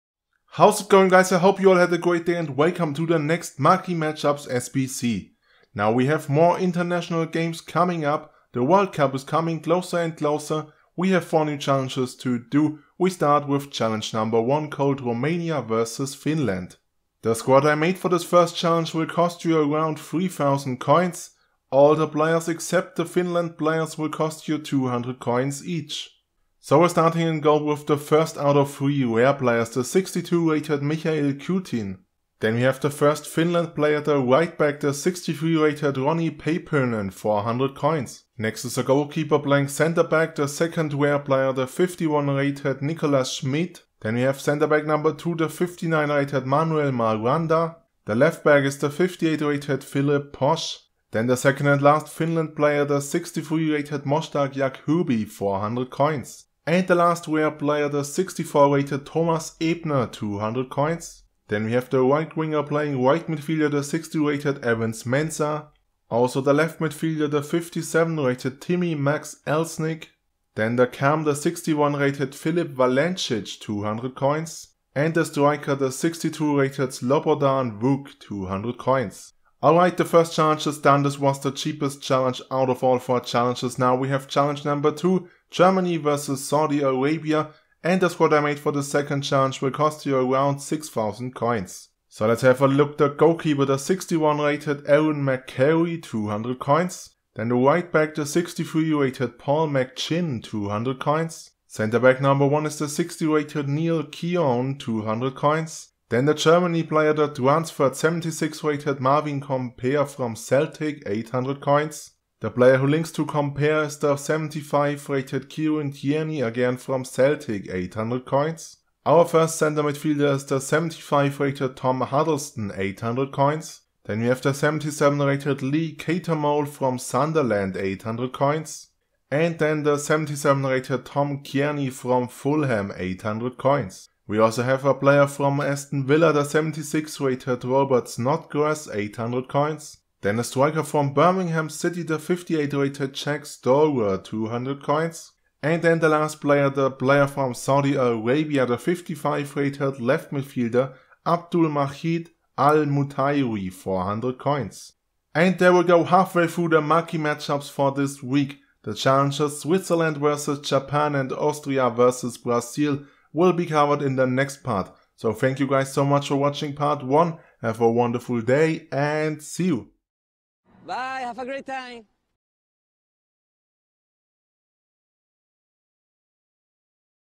MARQUEE MATCHUPS SBC - ROMANIA v FINLAND + GERMANY v SAUDI ARABIA (CHEAP+EASY)
MARQUEE MATCHUPS SBC - ROMANIA v FINLAND + GERMANY v SAUDI ARABIA

Hey Guys, it´s WagnerHD and welcome to the first part of the new Marquee Matchups SBC. As always we´re starting with the first 2 challenges.

REQUIREMENTS:

ROMANIA v FINLAND

- Romania or Finland Players: Min. 1
- Leagues: Min. 3
- Nationalities: Min. 3
- Rare Players: Min. 3
- Team Chemistry: Min. 80
- Number of players in the Squad: 11

REWARD = 1x Gold Pack (5.000 Coins)

Costs = around 3.000 Coins

GERMANY v SAUDI ARABIA

- Germany Players: Min. 1
- Saudi Arabia Players: Min. 1
- Leagues: Min. 3
- Team Overall Rating: Min. 72
- Team Chemistry: Min. 85

REWARD = 1x Jumbo Premium Gold Pack (7.500 Coins)

Costs = around 6.000 Coins

PSN-Gamertag: WGNR77